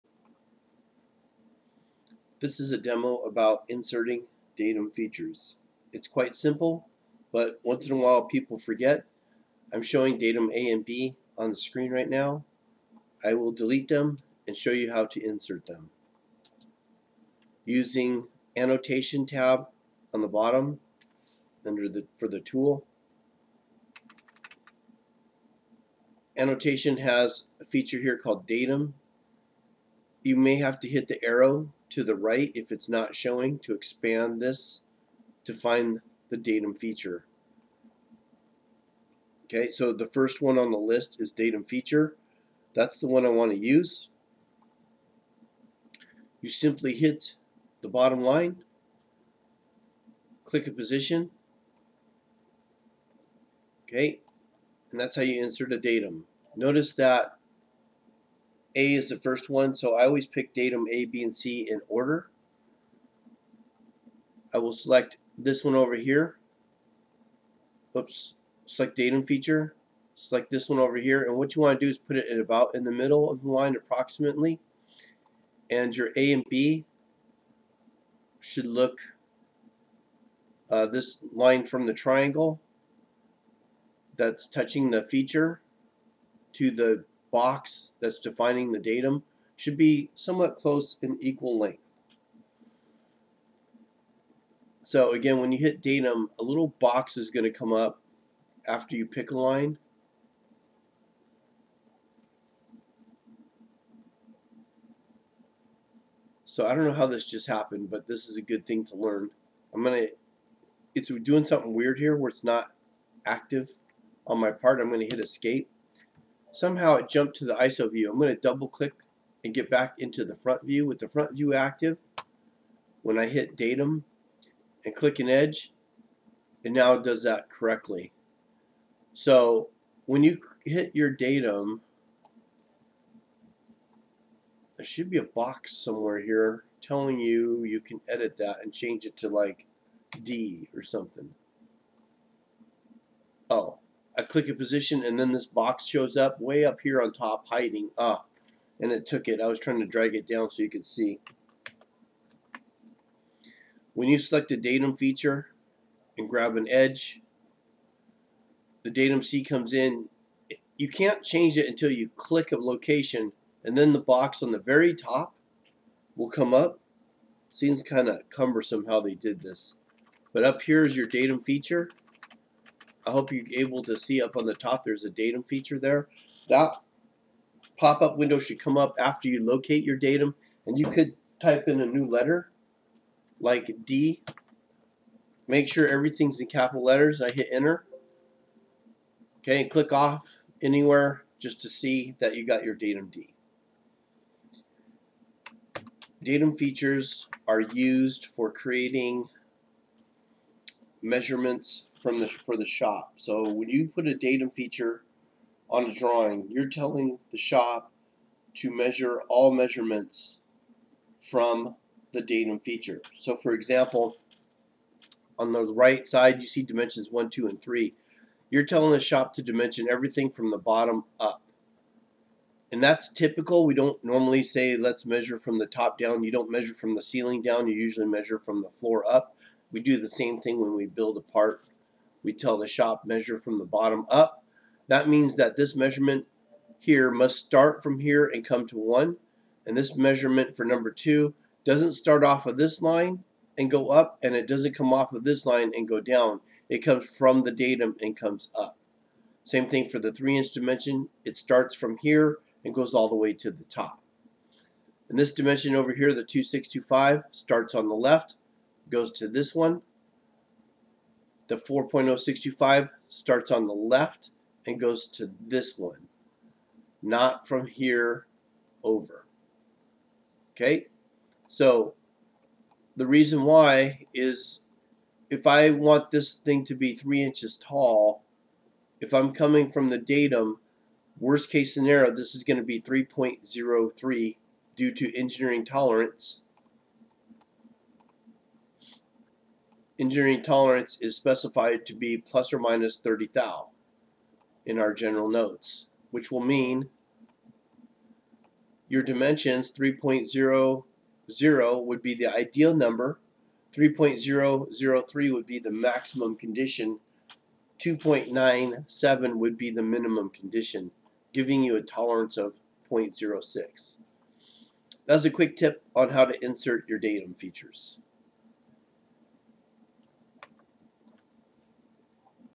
How to create datum features in a drawing CATIA 3D Experience
This video was created to help students currently enrolled at EvCC
Demo: How to create datum features in a drawing CATIA 3D Experience